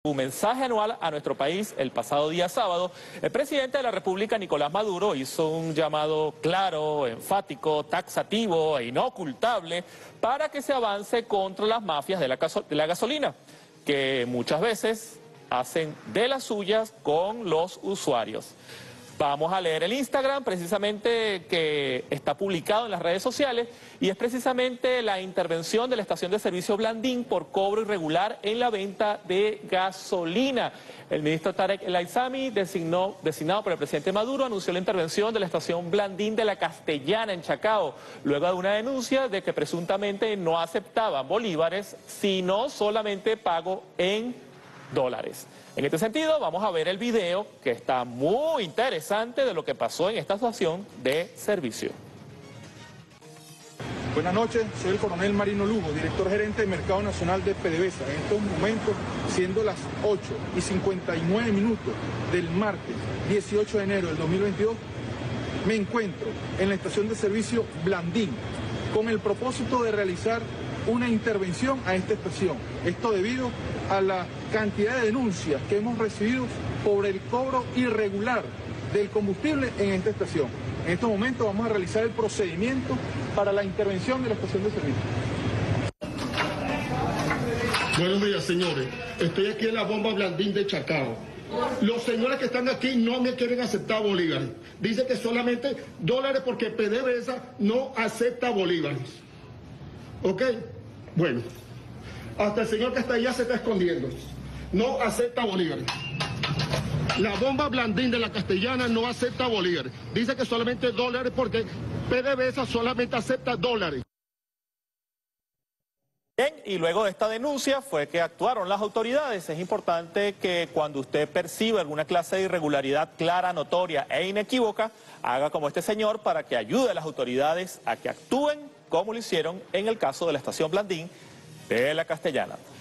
Intervienen estación de gasolina en Av Blandín de Chacao tras denuncias de que no acepta bolívares
El director gerente de Mercado Nacional de Pdvsa, por órdenes del ministro de Petróleo de Venezuela, Tareck El Aissami, intervino este martes 18 de enero de 2022 en la noche la estación de servicio de PDV en la Av. Blandín de Chacao, luego de que circularan denuncias de que en la misma presuntamente no se está aceptando pagos en bolívares, sino únicamente en dólares.

USDT o Bitcoin (BEP20): 0xef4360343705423a24119205eeb1de2a10fd23f4

USDT (TRC20): TQJB86JZjF5Kt1C7f322aax4iyD6x65UcV

Petros: PFfDUW3cj1WD7cUPYg9MK2EtgG5nUgVzuc